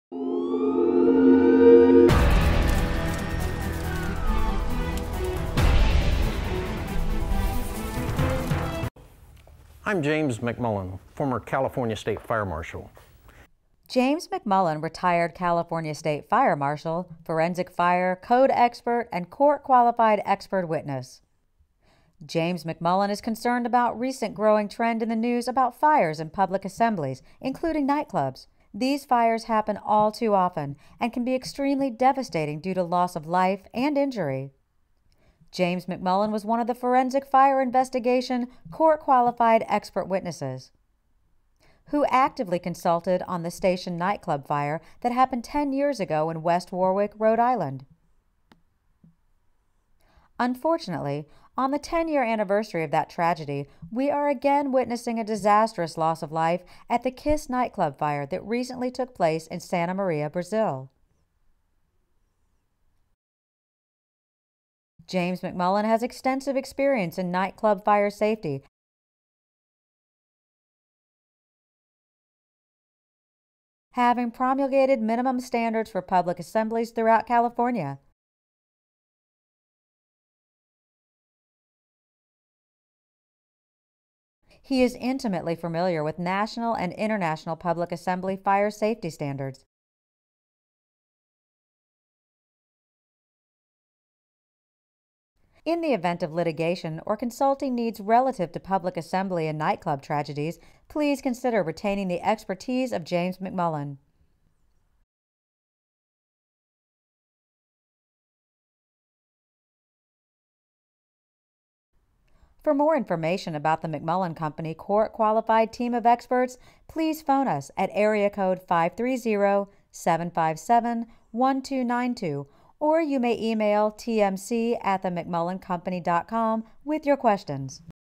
Nightclub Fire Court Qualified Expert Witness - Curriculum Vitae
- Nightclub Fire Expert Witness - Curriculum Vitae - James McMullen retired California State Fire Marshal, Forensic Fire, Code Expert and court qualified expert witness.

James McMullen is concerned about resent growing trend in the news about fires in public assemblies including nightclubs. These fires happen all too often and can be extremely devastating due to loss of life and injury.

James McMullen was one of the forensic fire investigation, court qualified expert witnesses.

Who actively consulted on the Station Nightclub fire that happened 10 years ago in West Warwick Rhode Island.

Unfortunately, on the 10 year anniversary of that tragedy we are again witnessing a disastrous loss of life at the Kiss Nightclub fire that recently took place in Santa Maria, Brazil.

James McMullen has extensive experience in nightclub fire safety having promulgated minimum standards for public assemblies throughout California. He is intimately familiar with national and international public assembly fire safety standards. In the event of litigation or consulting needs relative to public assembly and nightclub tragedies, please consider retaining the expertise of James McMullen.

For more information about The McMullen Company court qualified team of experts.